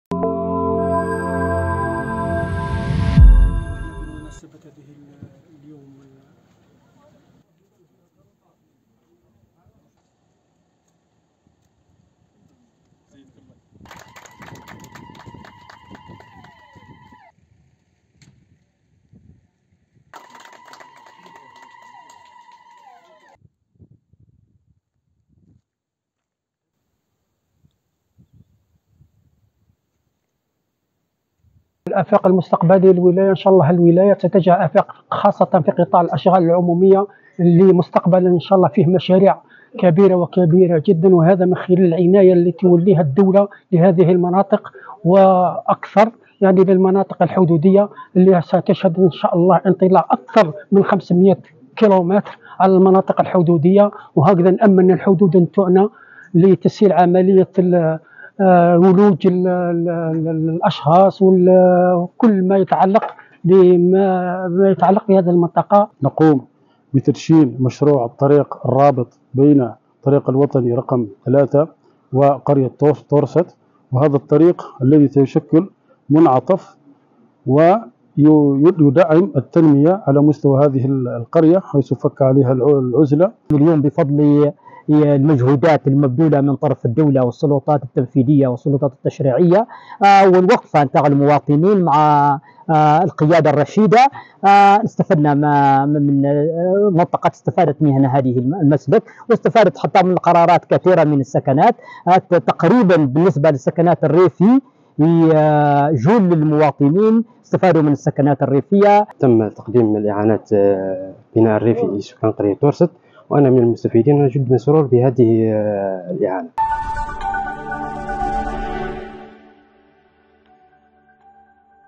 جانت: وضع حيز الخدمة لمشاريع تنموية وتوزيع مقررات استفادة من إعانات البناء الريفي
جانت(وأج)- تزامنا مع الاحتفالات المخلدة للذكرى الـ 62 لعيد الاستقلال، تدعمت ولاية جانت، بمشاريع تنموية تخص قطاع الأشغال العمومية وتوزيع مقررات استفادة من إعانات البناء الريفي ببلدية برج الحواس. ويتعلق الأمر بوضع حيز الخدمة للطريق الولائي رقم 474 الرابط بين قرية إهرير النائية (240كم شمال جانت) والطريق الوطني رقم 3 على مسافة 15 كم بعد عصرنته وتجديده كليا ووضع حيز الخدمة الطريق الرابط بين قرية تورست النائية والطريق الوطني رقم 3 على مسافة 44 كم كما استفادت منطقة تورست النائية من توزيع قرارات إعانة للبناء الريفي